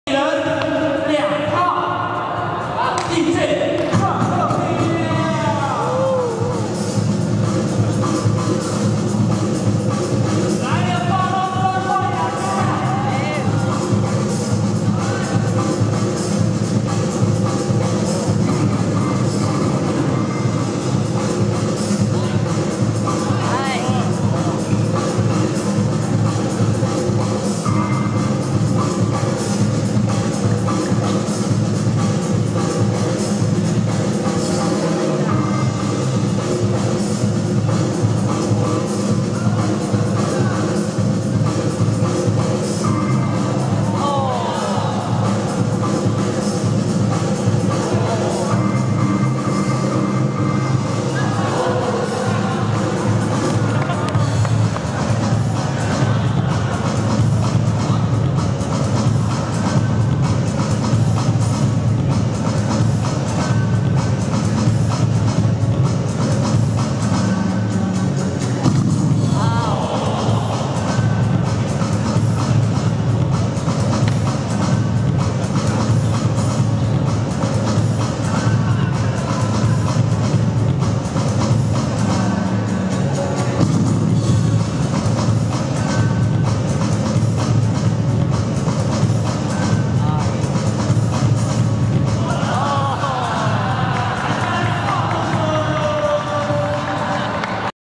2015 RAIN CITY BATTLE VOL 5 POWER MOVE FINAL Power Sour VS 小鬼
RAIN CITY BATTLE
KEELUNG CITY